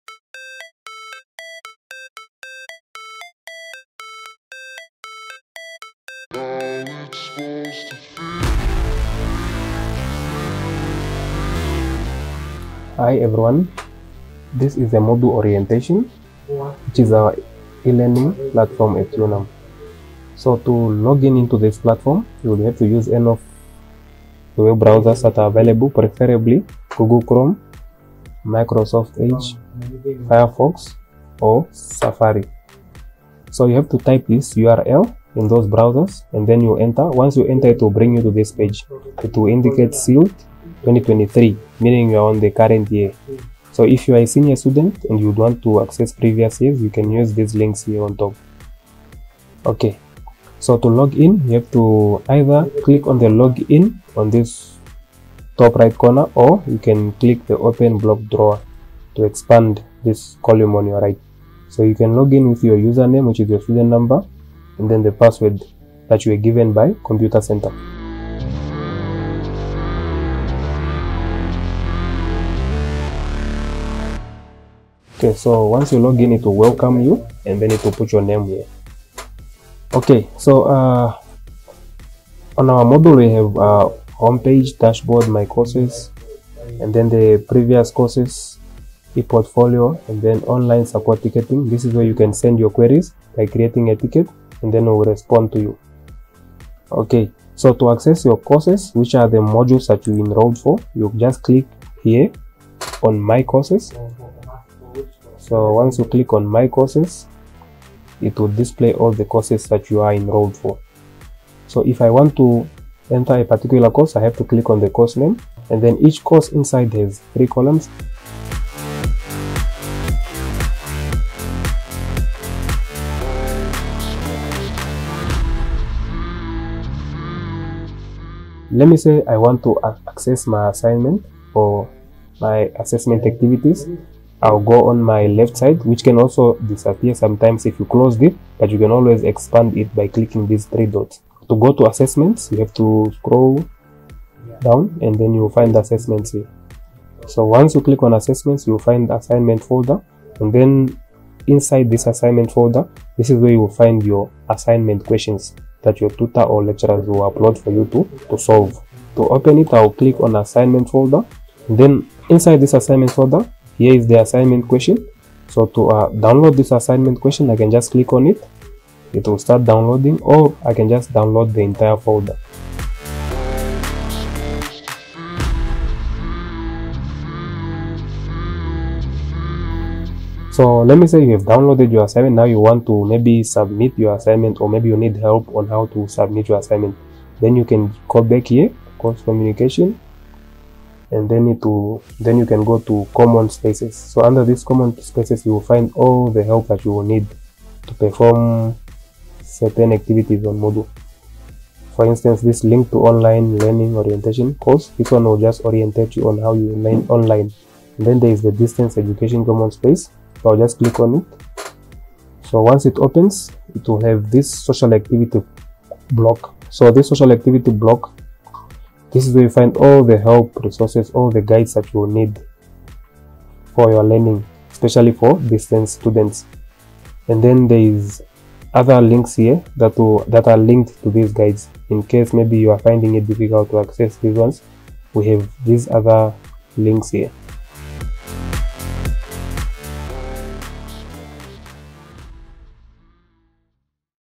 CILT Quick Bite - What you need to know in Moodle 2023
What you need to know in Moodle 2023 is a short tutorial guiding students through topics such as:
00:00 - Start
00:07 - Logging in
01:12 - Finding your courses
02:02 - How to find your assignments
02:54 - How to get more assistance - quick links